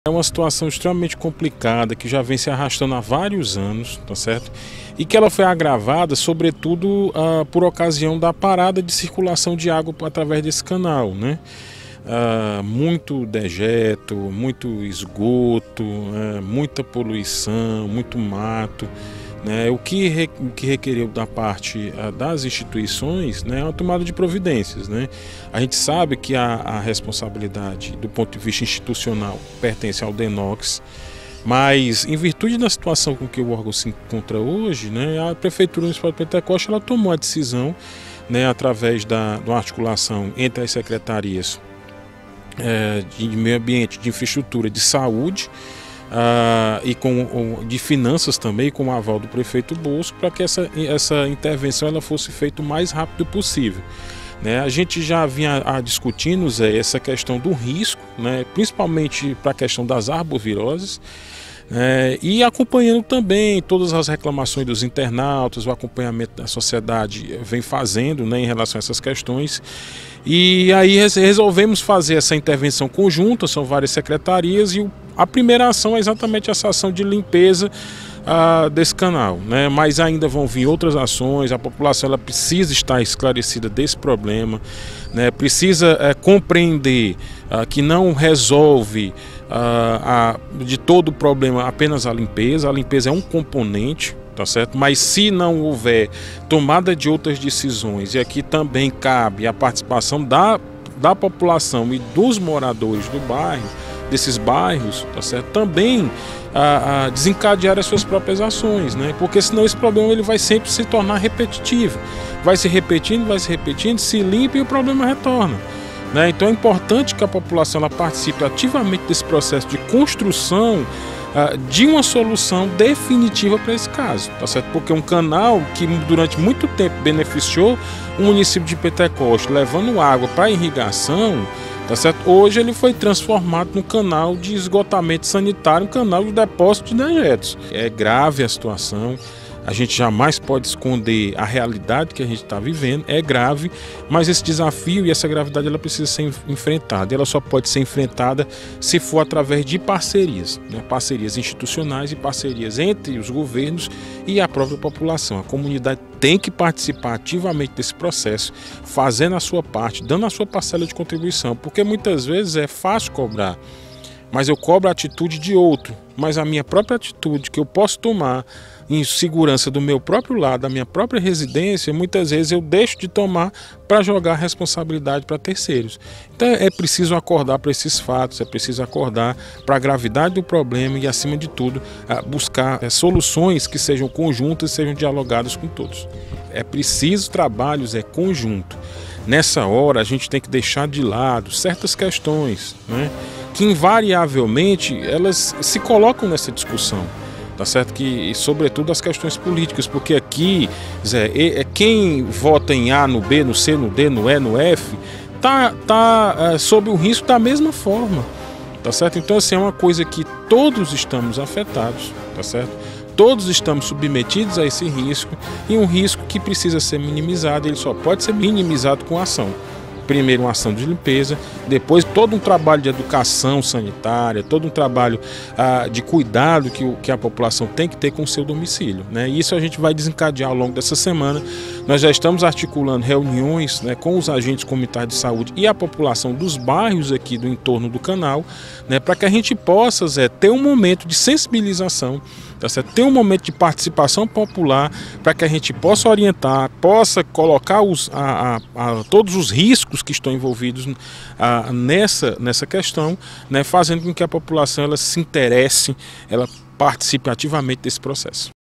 Prefeitura de Pentecoste faz limpeza do canal do Dnocs no bairro Planalto
Após a reivindicação de populares, abordagem no Programa Pentecoste em Foco exibido pelo Notícias de Pentecoste, e de cobranças na Câmara Municipal feita pelos vereadores Hailton Castro e Tiago Castro, a Prefeitura de Pentecoste, por meio da Secretaria de Infraestrutura e Desenvolvimento Urbano e da Secretaria de Meio Ambiente e Urbanismo, iniciou nesta quinta-feira (01) o trabalho de limpeza do canal do Departamento Nacional de Obras Contra as Secas (DNOCS), no bairro Planalto. O secretário de Meio Ambiente de Pentecoste, Daniel Gomes, explica em vídeo como está sendo feito o trabalho de limpeza para evitar a proliferação de doenças. Acompanhe:

Por André Barros
Editor do Blog Notícias de Pentecoste